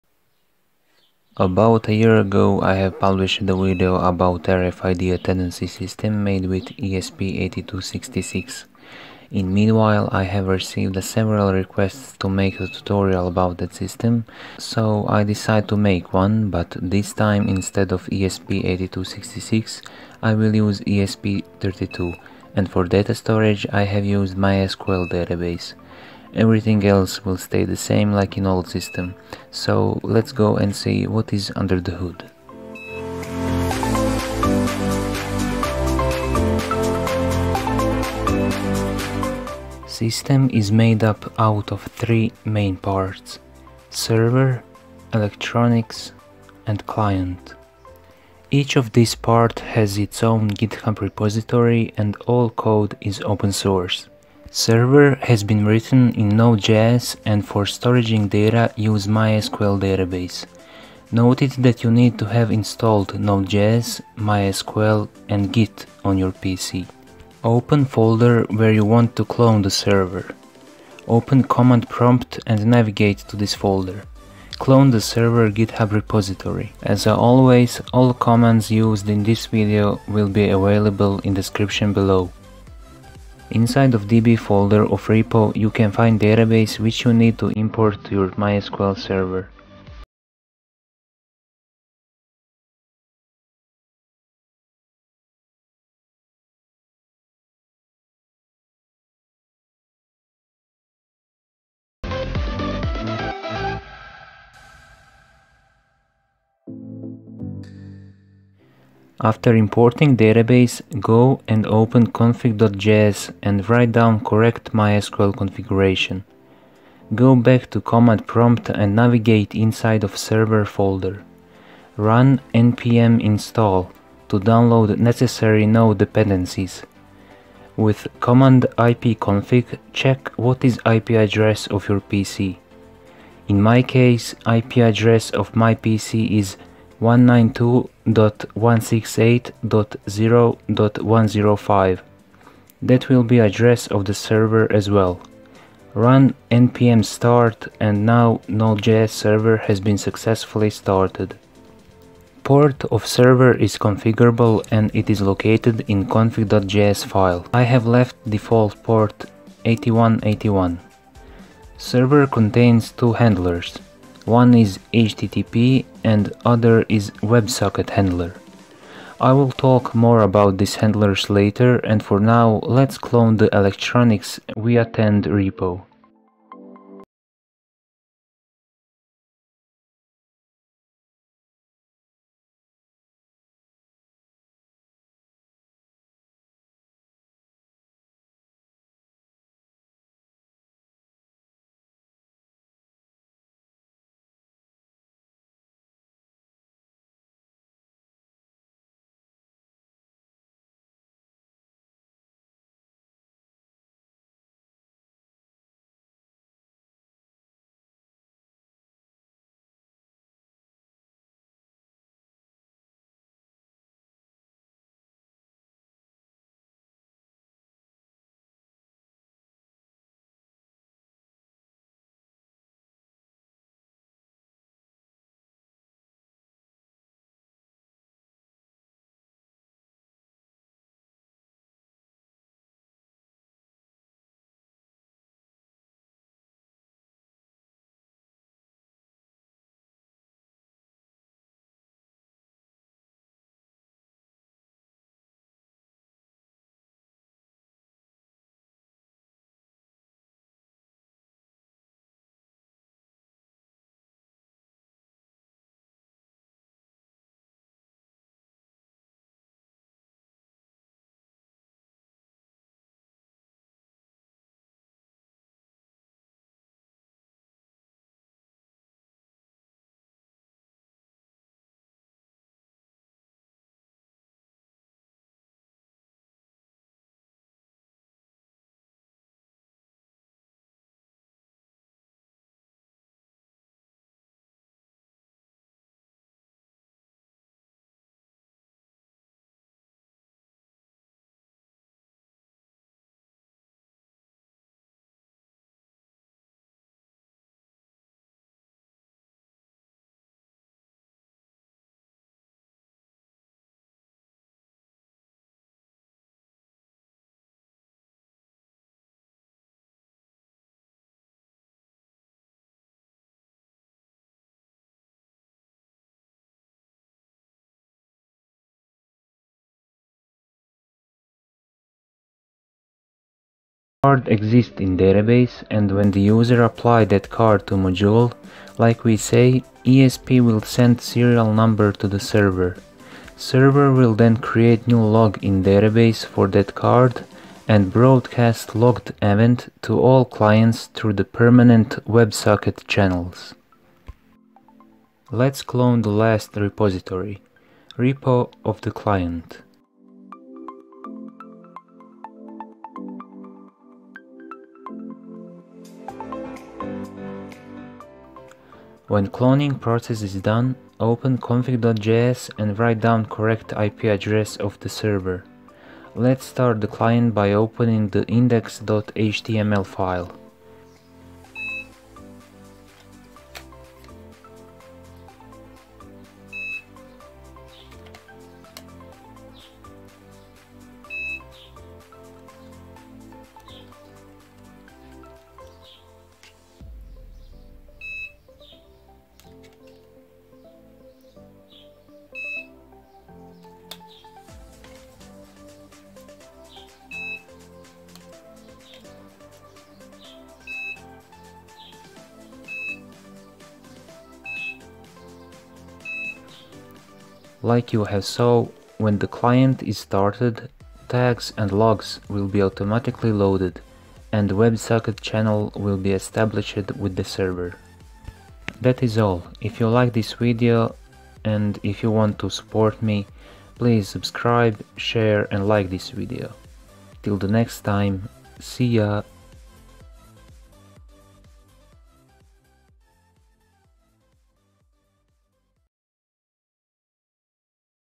RFID Attendance System - ESP32 - NodeJS + MySQL
Tutorial. RFID Attendance System realized using ESP32 and RC522 RFID module. System use NodeJS for serving and MySQL database for storing data.